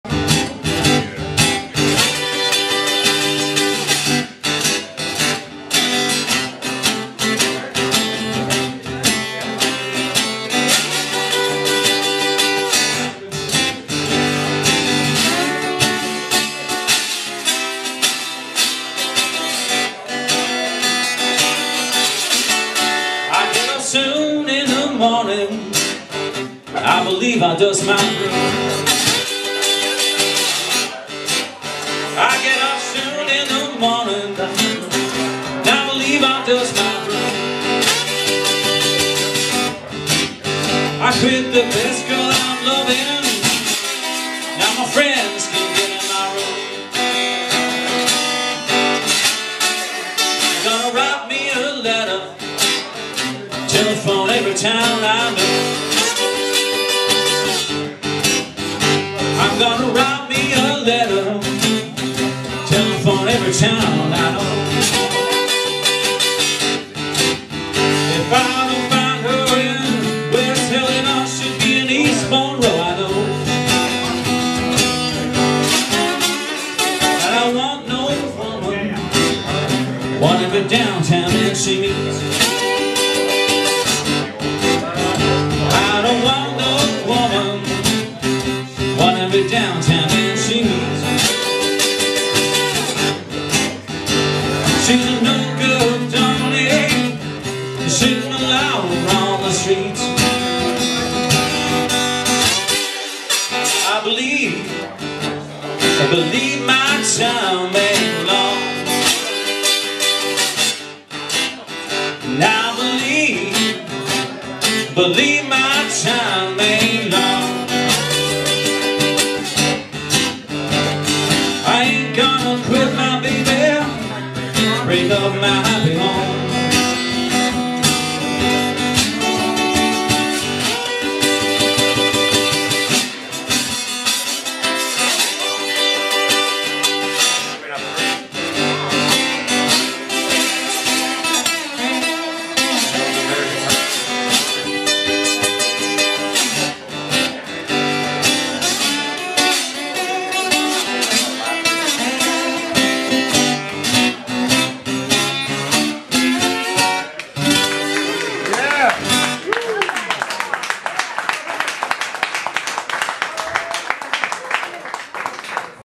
Pete Webb acoustic
the very talented Pete Webb at Brasserie Unik's acoustic night special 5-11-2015.
(sorry about the picture quality , camera wasn't on the right setting)